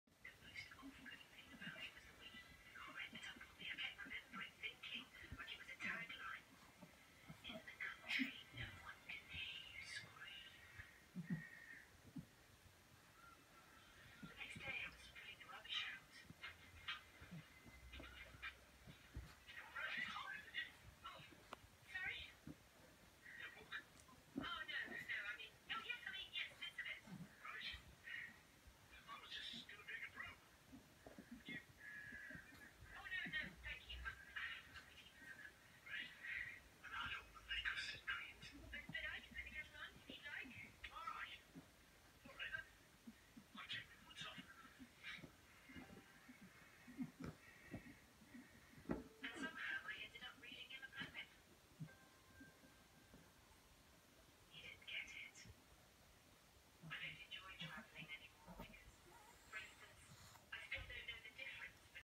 Bed too big, Chihuahua can’t get comfortable
Izz attempting to get comfortable in a greyhound’s bed.